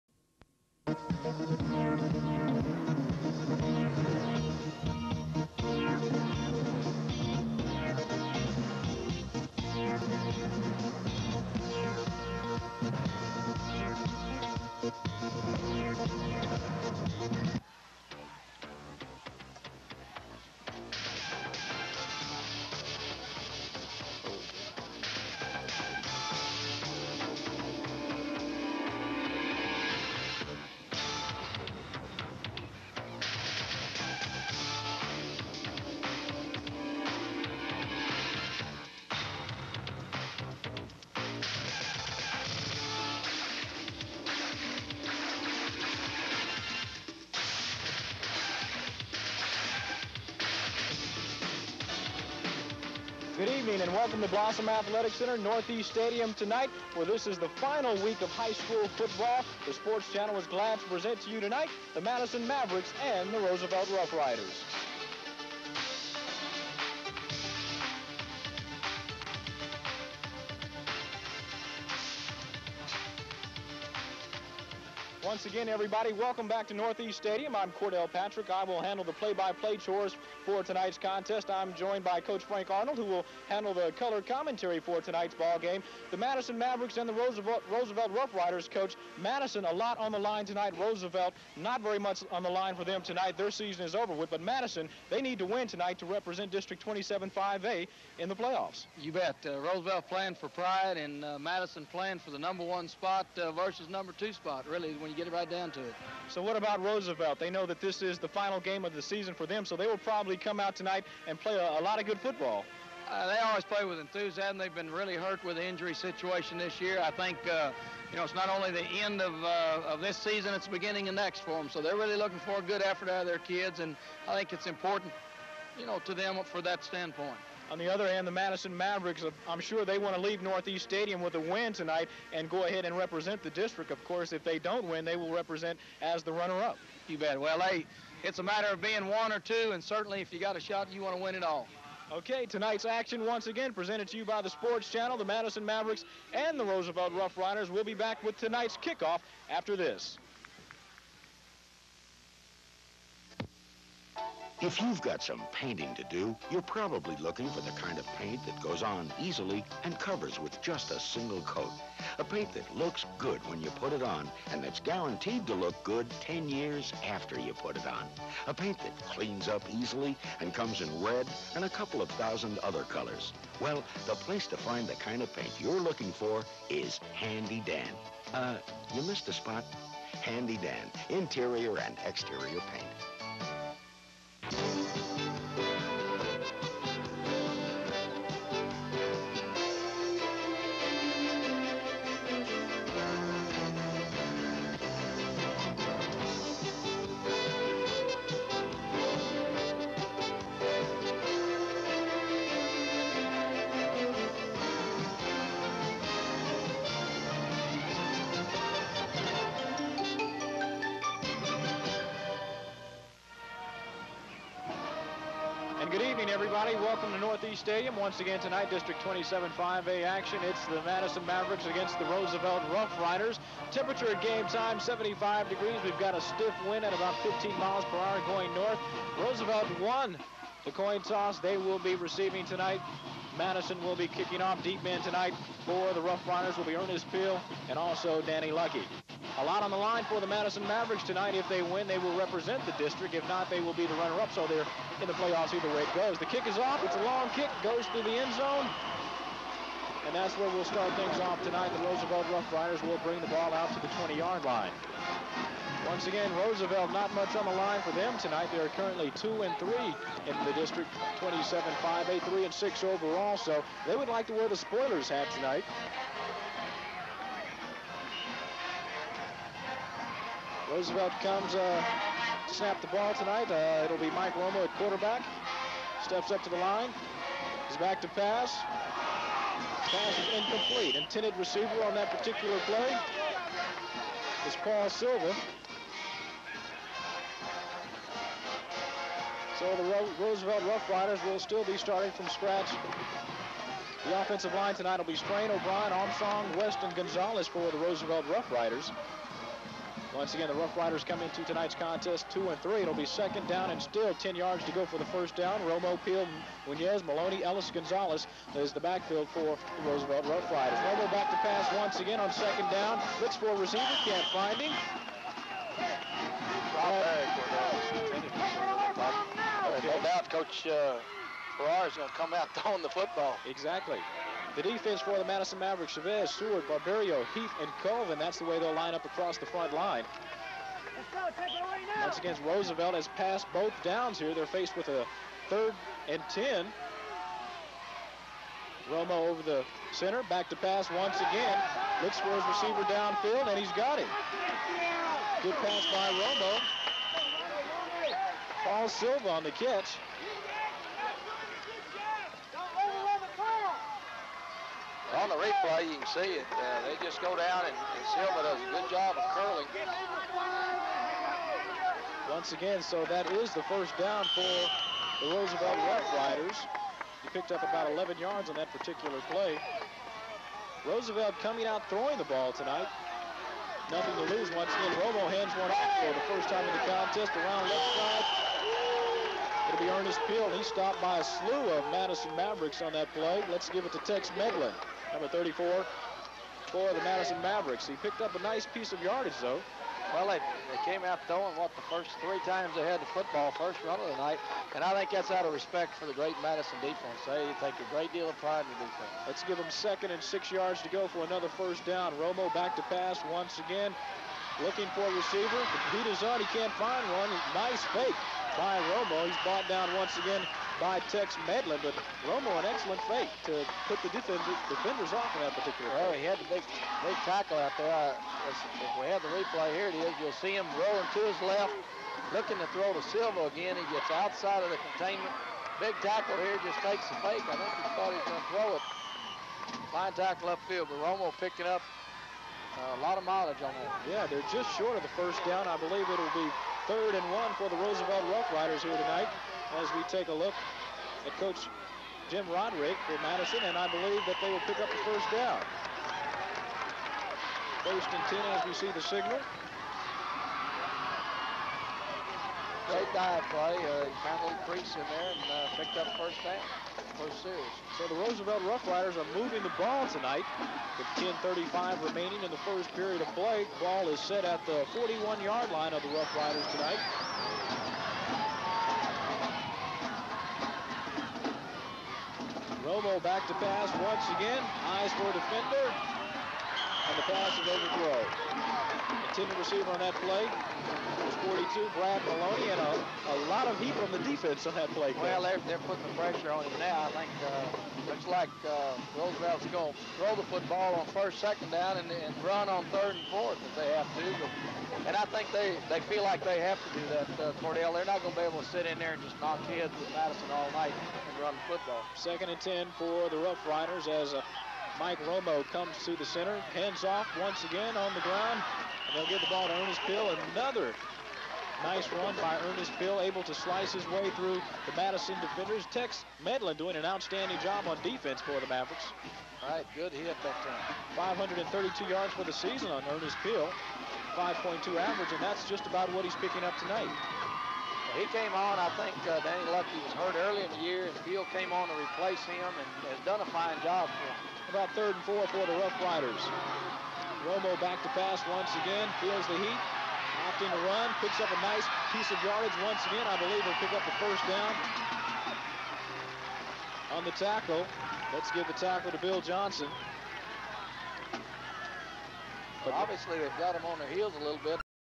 1985 San Antonio Roosevelt vs San Antonio Madison 1st qtr part 1
Broadcast of 1985 high school football San Antonio Roosevelt vs San Antonio Madison. Madison won 30-13.